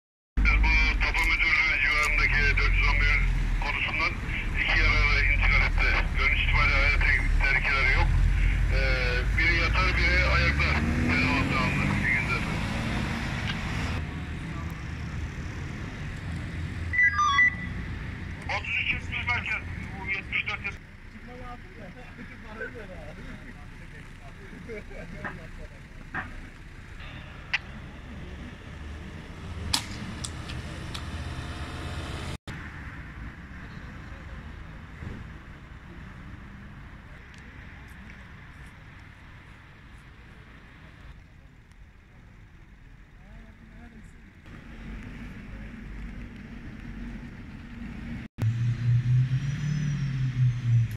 Kahramanmaraş’ta ortalık savaş alanına döndü
Kahramanmaraş’ta iki otomobilin çarpışması sonucu meydana gelen trafik kazasında 3 kişi yaralandı.

Kahramanmaraş Manşet Gazetesi